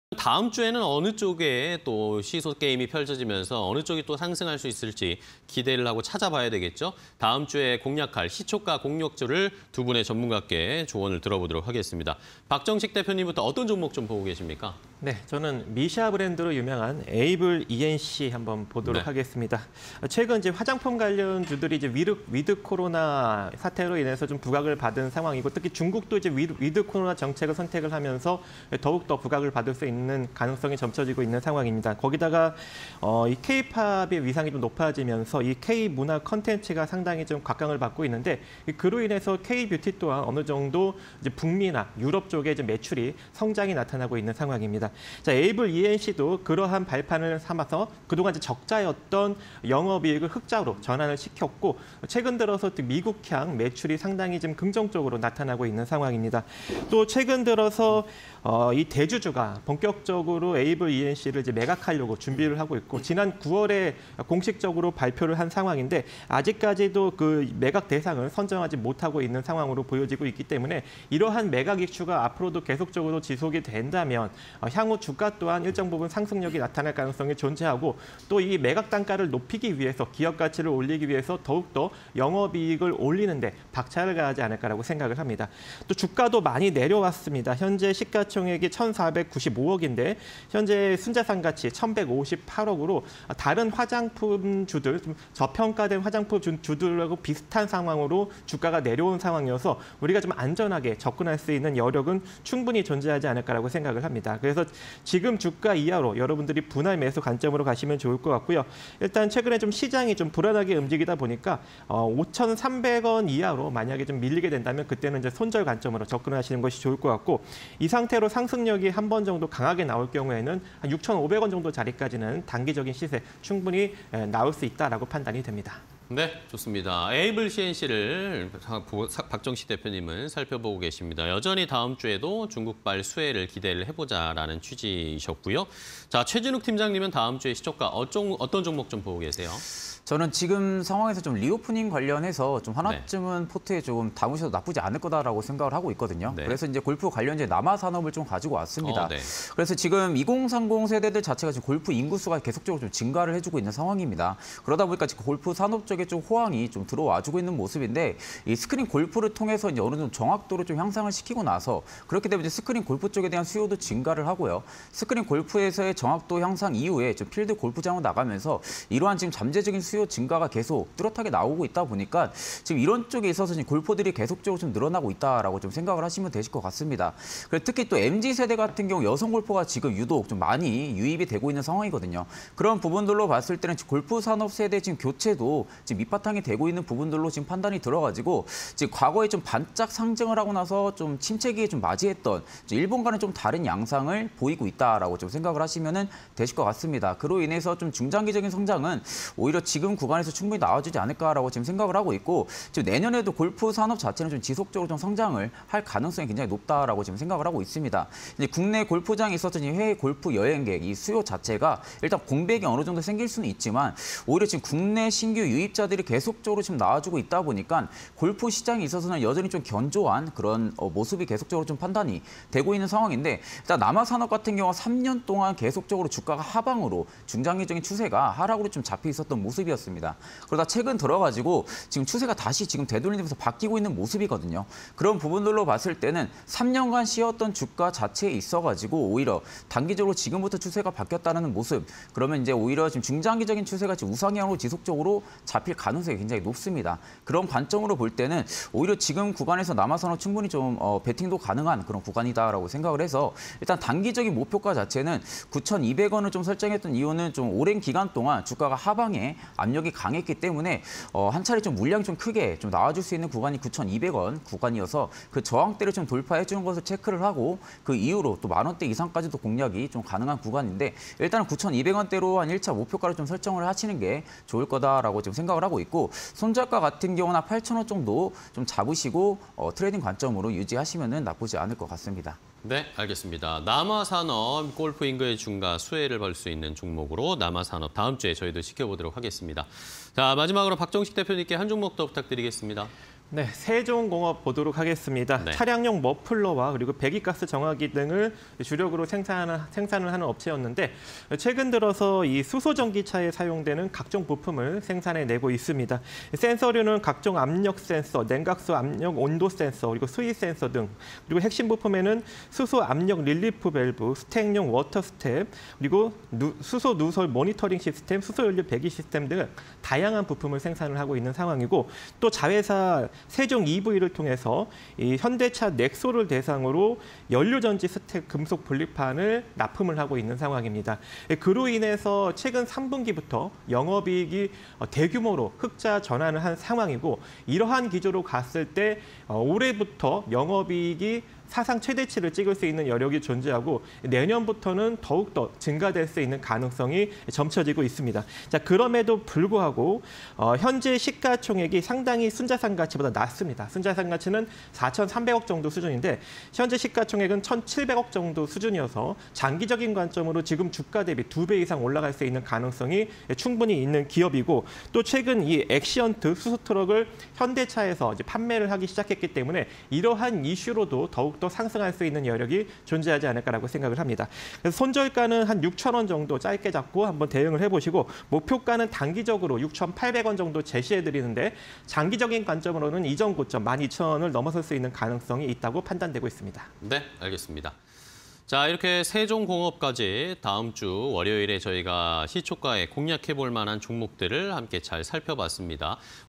에이블씨엔씨(078520), 남화산업(111710), 세종공업(033530)_사가지 (20221209)
에이블씨엔씨(078520)
- 화장품ㆍ생활용품 관련 사업 영위
- 화장품 브랜드 '미샤(MISSA)' 보유… 현지화 전략 추진
- 인수금융 만기 연장 못해… 매각 이슈 지속

남화산업(111710)
- 광주ㆍ전남권 최대 골프장 운영… 높은 수익성
- 가장 낮은 그린피에도 높은 수익성
- 국내 신규 유입자로 인해 수익 견고할 전망

세종공업(033530)
- 차량용 머플러(소음기) 등 자동차 부품 제조 업체
- 수소ㆍ전기차 센서류 및 핵심부품 생산
- 현대차 '넥쏘' 대상으로 금속분리판 납품
- 현대차 '엑시언트' 수소트럭 판매 개시